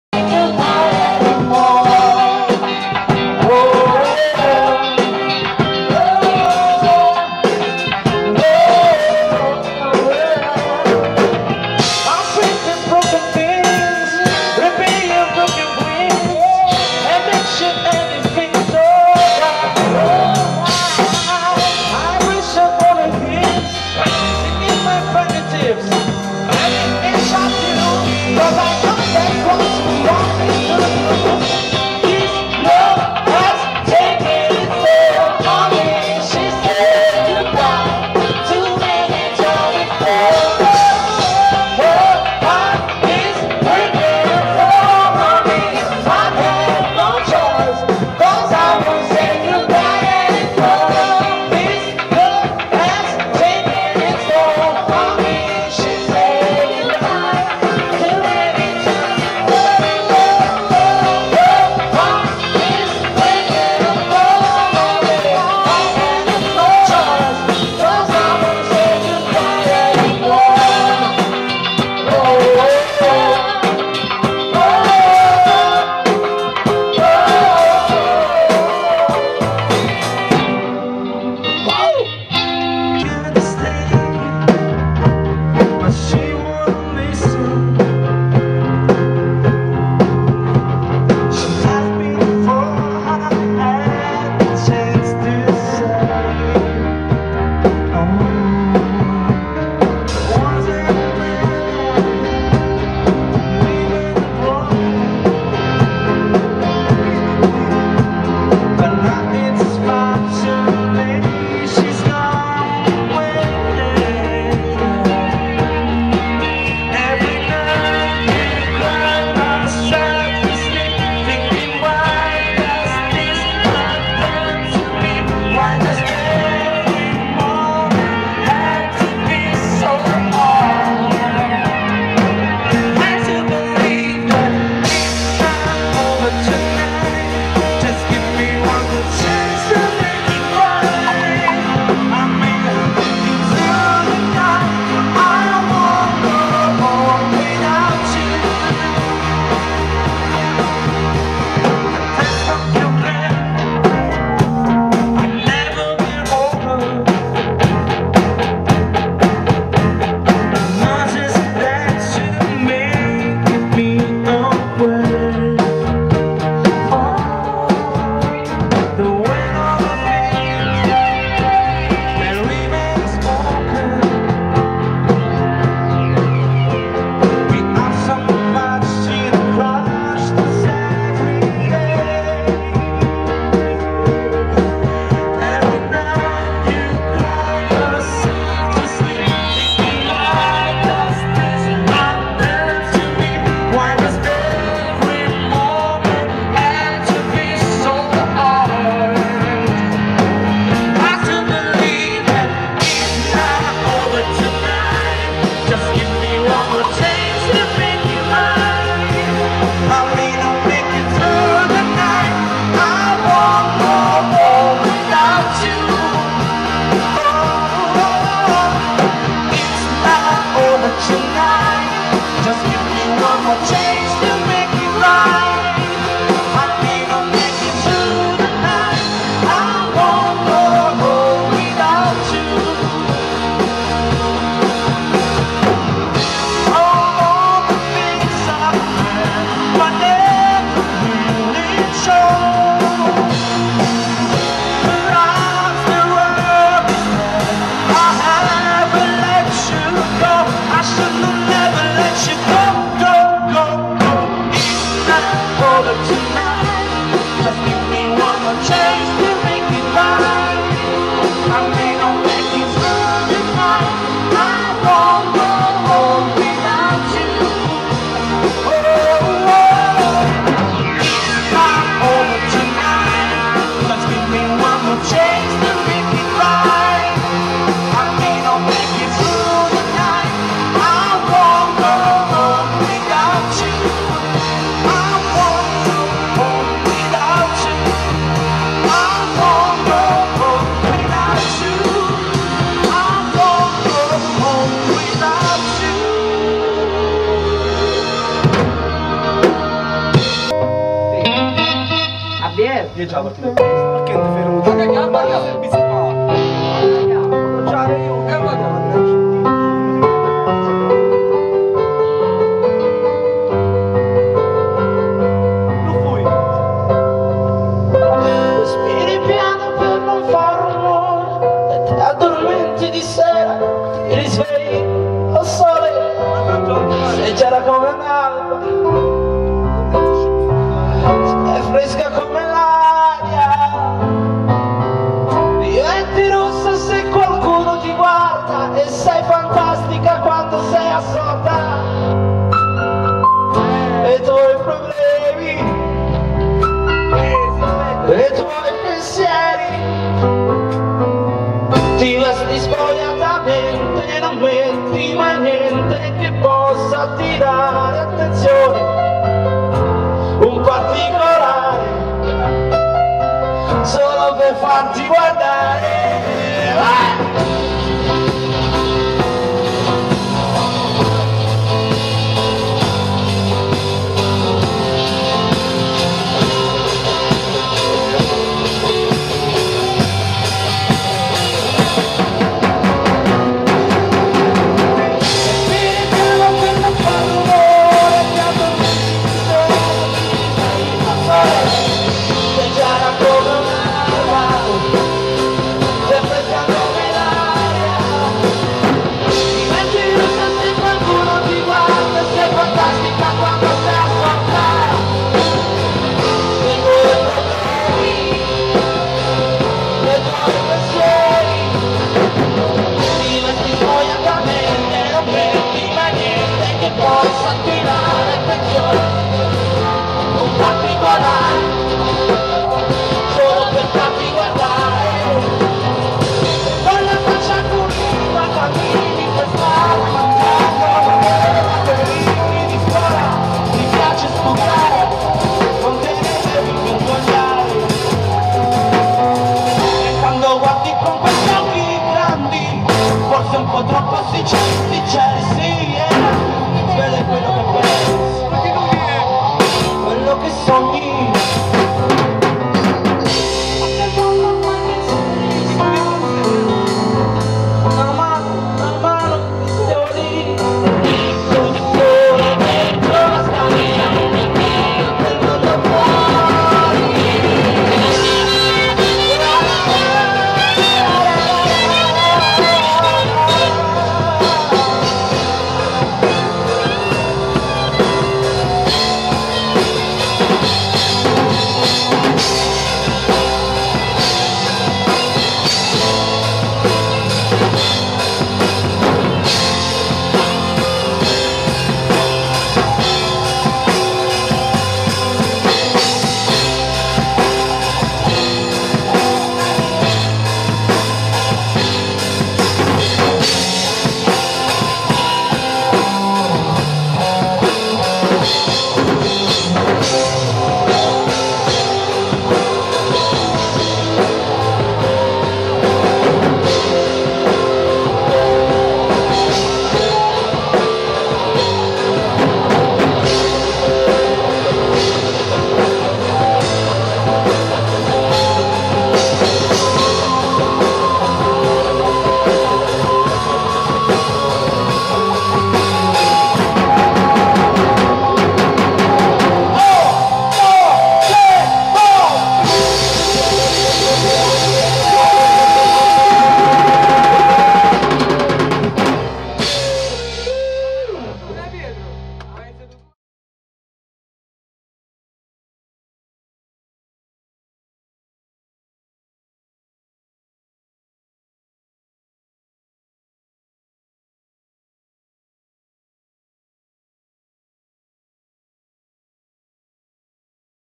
Villa Literno Eject in prova
eject è un gruppo musicale emergente di villa literno in cerca di amici e serate